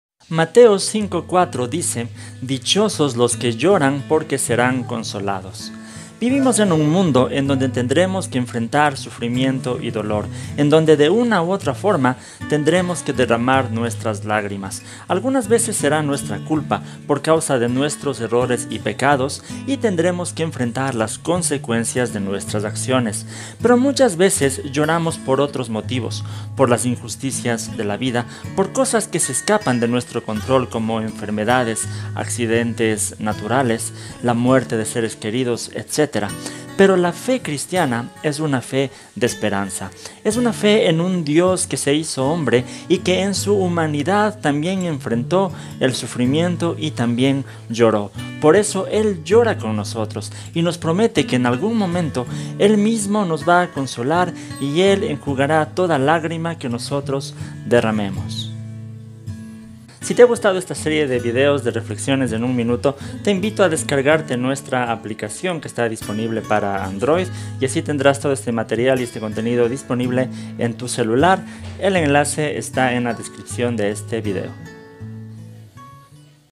😥Bienaventurados LOS QUE LLORAN - ⏱Reflexiones en UN MINUTO - Mateo 5:4 - Canal Cristiano - TodoTeos
Una corta reflexión sobre la segunda Bienaventuranzas

Mis libros en Amazon

Mi libro La Búsqueda de Jaris